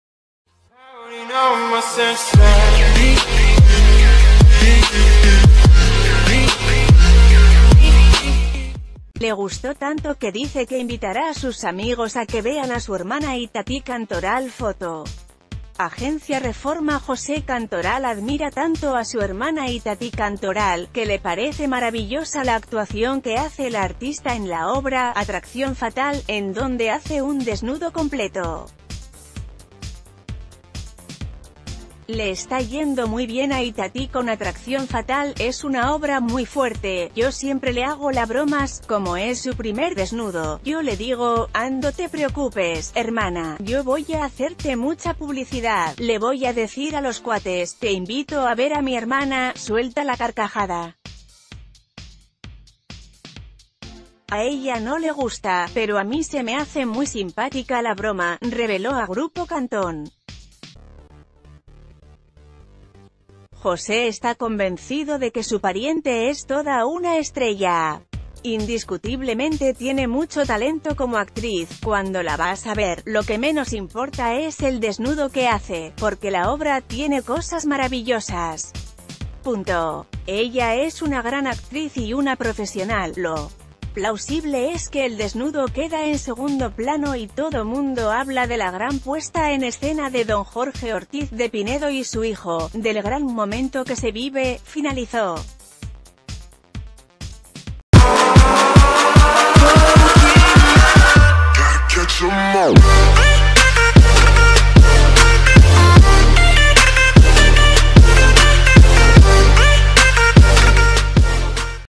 Hermano de Itatí Cantoral celebra su desnudo
Le gustó tanto que dice que invitará a sus amigos a que vean a su hermana Itatí Cantoral Foto: Agencia Reforma José Cantoral admira tanto a su hermana Itatí Cantoral, que le parece maravillosa la actuación que hace la artista en la obra “Atracción fatal”, en donde hace un desnudo completo. “Le está ...